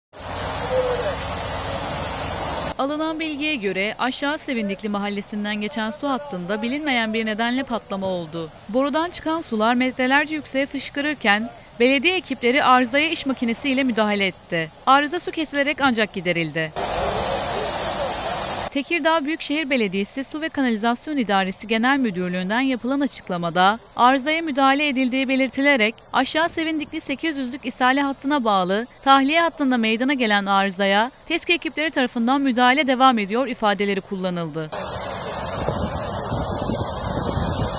PATLAYAN BORUDAN SULAR METRELERCE HAVAYA YÜKSELDİ
Tekirdağ'ın Muratlı ilçesinde su tahliye hattında meydana gelen patlamanın ardından borudan çıkan sular metrelerce yükseğe fışkırdı.